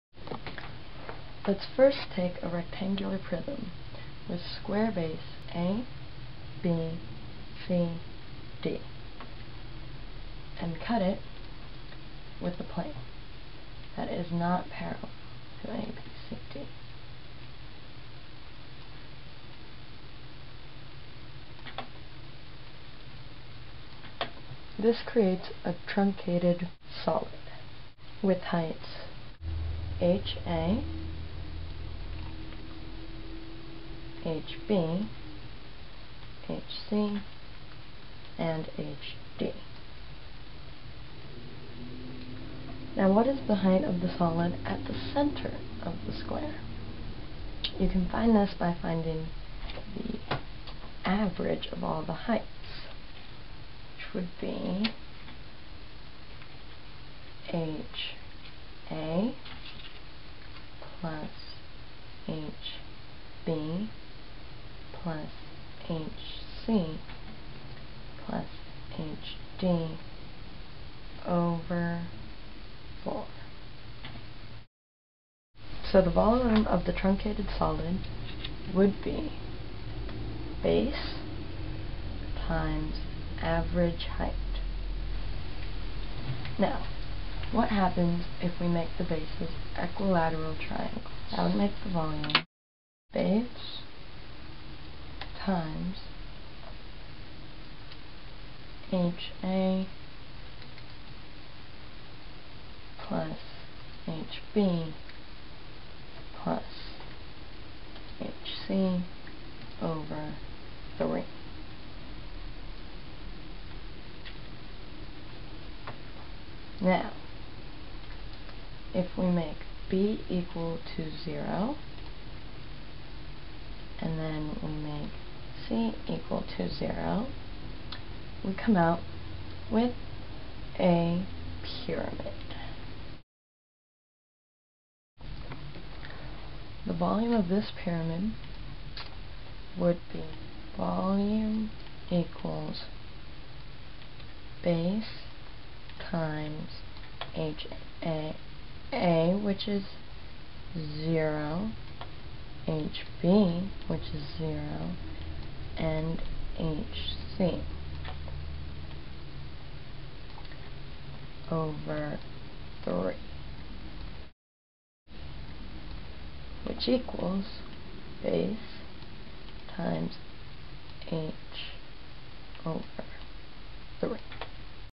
Why is the volume of a pyramid 1/3bh
Why is the volume of a pyramid 1/3 bh or bh/3 why isn't it just bh or 1/2 or 1/4?